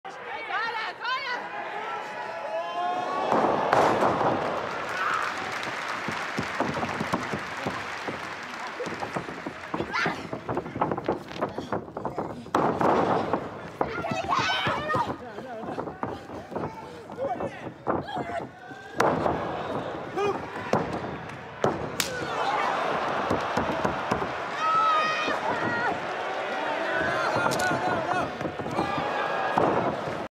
【女子プロレス】安納サオリが鈴季すずに雪崩式フランケンシュタイナー！【スターダム】
◆テレビ番組
他

▽選手Twitter
◆STARS